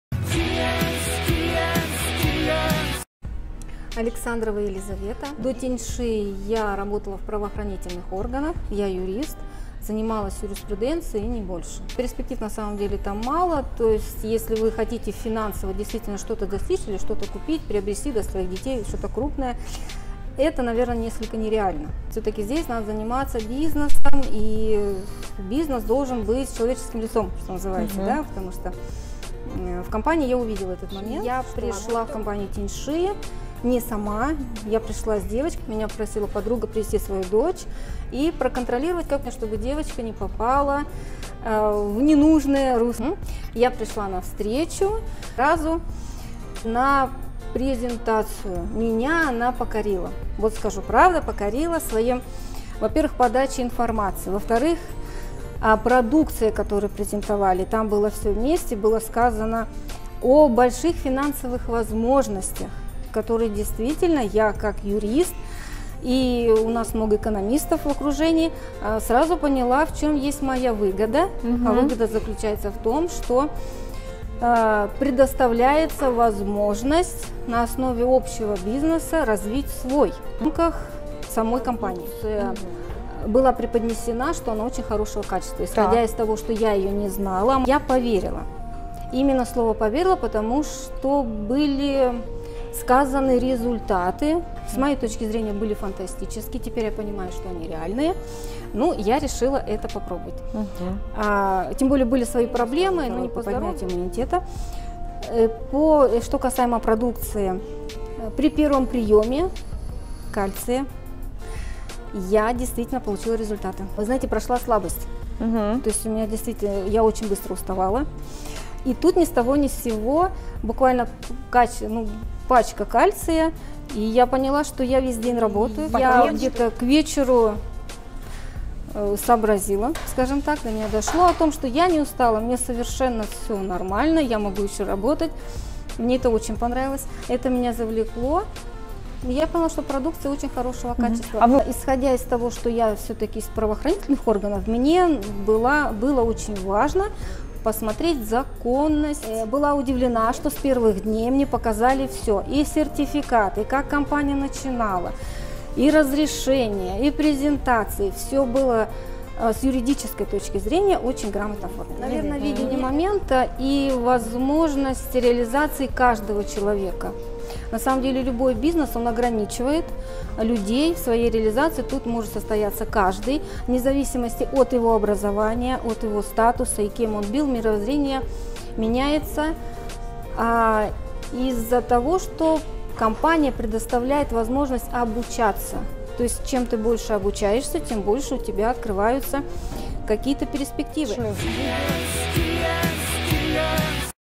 45 лет Юрист
45 лет, г. Москва. До Тяньши Елизавета Александровна работала в правоохранительных органах -  юристом. Для себя поняла, что перспектив там мало, и достичь финансового успеха нереально, и поняла, что для этого нужно заниматься бизнесом. Пришла на встречу в компанию, и сразу посетила презентацию. Елизавету Александровну презентация покорила подачей информации, о продукции компании, о финансовых возможностях, которые можно достигнуть, сотрудничая с компанией. И увидела для себя выгоду, а именно вести свое дело в рамках компании. Также у Елизаветы Александровны был ослабленный иммунитет, и ее заинтересовал продукт. Начала принимать общий кальций, и обнаружила что у нее прошла слабость, усталость. Поняла для себя что она полна сил и энергии. Исходя из того, что Елизавета из правоохранительных органов, ей была важна законность компании. Была приятно удивлена, что с первых дней показали сертификаты, разрешения, и историю компании и всё было с юридической стороны грамотно оформлено. Для себя увидела, что в компании может состояться каждый, независимо от возраста, профессии и знаний. И увидела для себя много перспектив, а о них Елизавета расскажет в ролике.